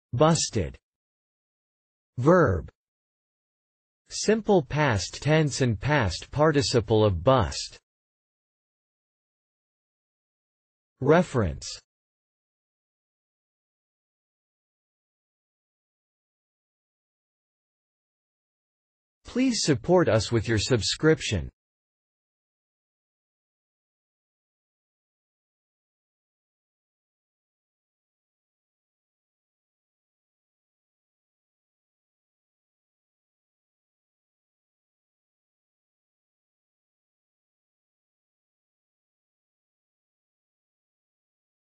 Busted | Meaning of busted 📖
See here, the meanings of the word busted, as video and text.

busted (verb)
    Simple past tense and past participle of bust.

Reference:
    busted